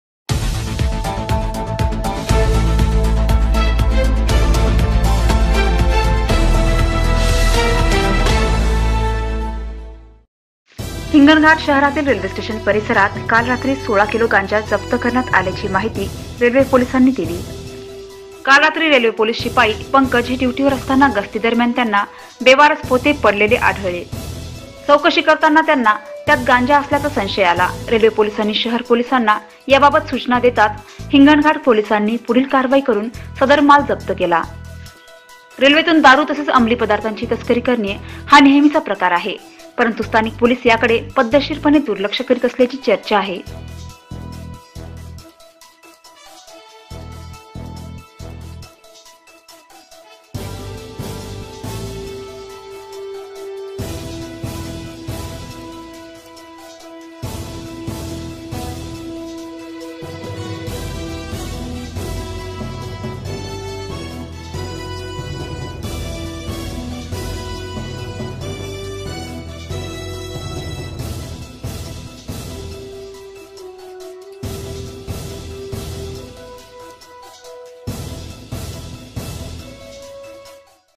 हिंगणघाट शहरातील रेल्वे स्टेशन परिसरात सोळा किलो गांजा जप्त...
हिंगणघाट दि.३० आगस्ट: शहरातील रेल्वे स्टेशन परिसरात काल रात्री सोळा किलो गांजा जप्त करण्यात आल्याची माहिती रेल्वे पोलिसांनी दिली.
  काल रात्री रेल्वे पोलिस शिपाई पंकज हे ड्यूटीवर असतांना गस्ती दरम्यान त्यांना बेवारस पोते पडलेले आढळले,  चौकशी करता त्यांना त्यात गांजा असल्याचा संशय आला.
     रेल्वे पोलिसांनी  शहर पोलिसांना याबाबत सूचना देताच हिंगणघाट पोलिसांनी पुढील कारवाई करुन सदर माल जप्त केला.
       रेल्वेतुन दारू तसेच अंमली पदार्थांची तस्करी करने हा नेहमीचाच प्रकार आहे. परंतु स्थानिक पोलिस याकडे पद्धतशिरपणे दुर्लक्ष करीत असल्याची चर्चा आहे.